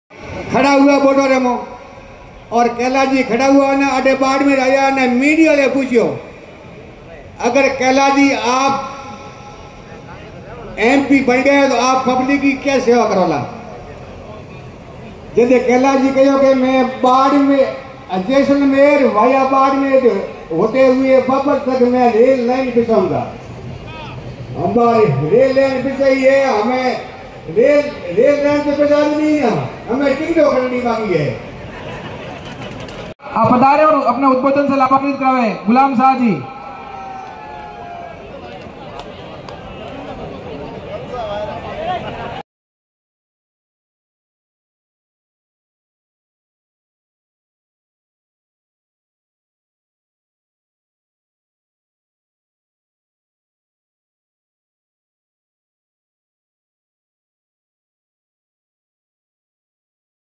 लोकसभा चुनाव 2024 बाड़मेर जैसलमेर कांग्रेस प्रत्याशी उम्मेदाराम बेनीवाल की नामांकन रैली में जनसैलाब।
लोकसभा चुनाव 2024 में बाड़मेर जैसलमेर से कांग्रेस प्रत्याशी उम्मेदाराम बेनीवाल की  बाड़मेर के आदर्श स्टेडियम में नामांकन रैली में उमड़ा जनसैलाब।